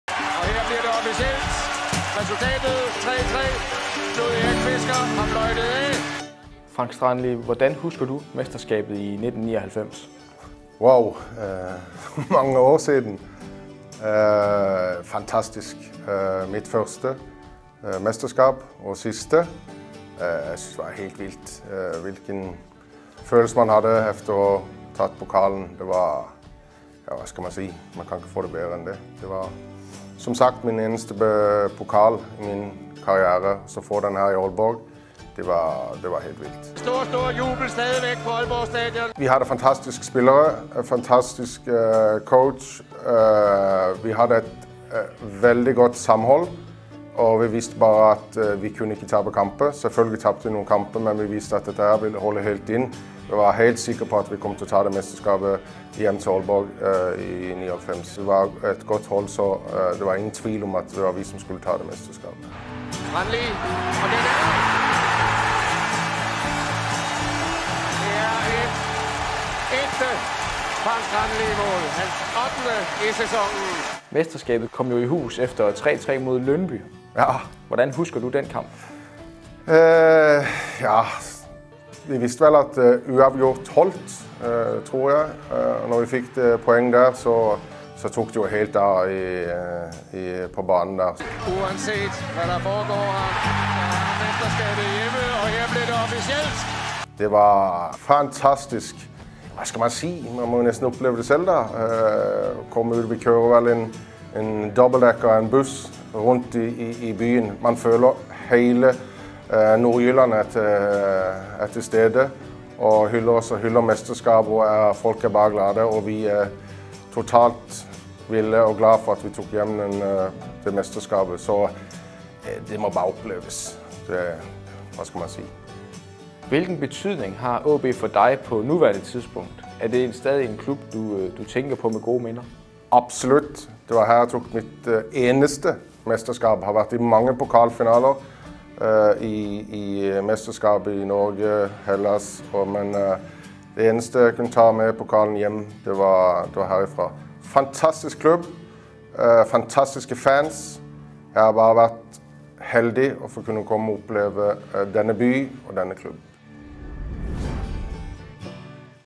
Frank Strandli om AaB's mesterskab i 1999
I anledning af 20-års jubilæet for mesterskabet i 1999 har AaB TV talt med nordmanden Frank Strandli om guldsæsonen, hvori han spillede 17 kampe og scorede ni mål.